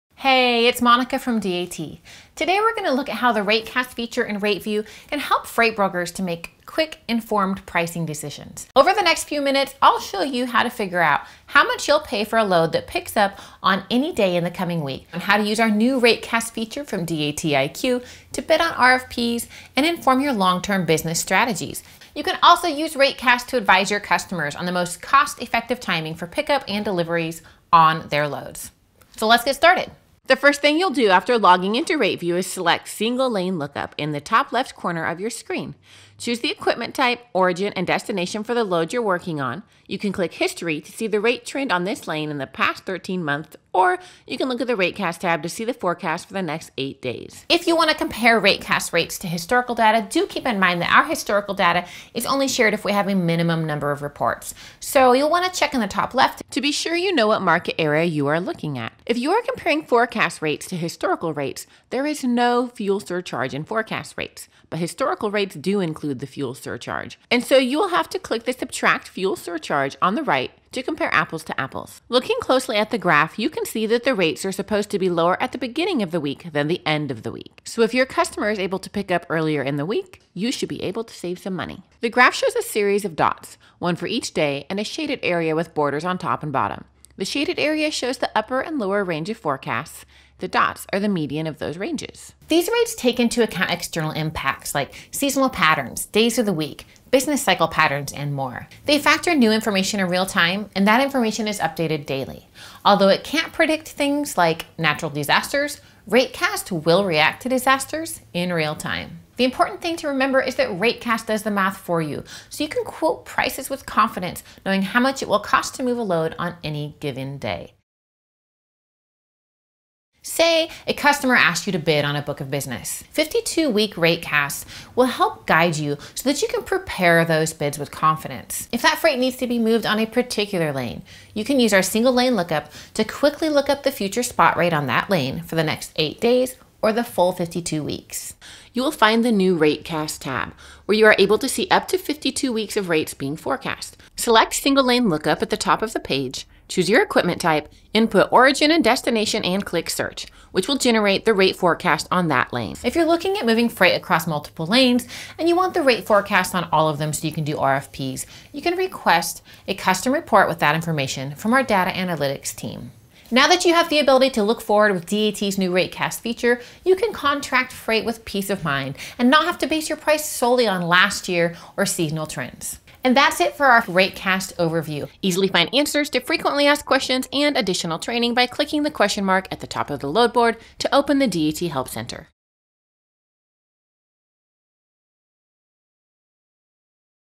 Ratecast Full Overview For Review
How to Use DAT's new Rateview feature, Ratecast. Learn how to plan your week's shipments with 8-day Ratecast. And how to bid on RFP's with insight using 52-week Ratecast.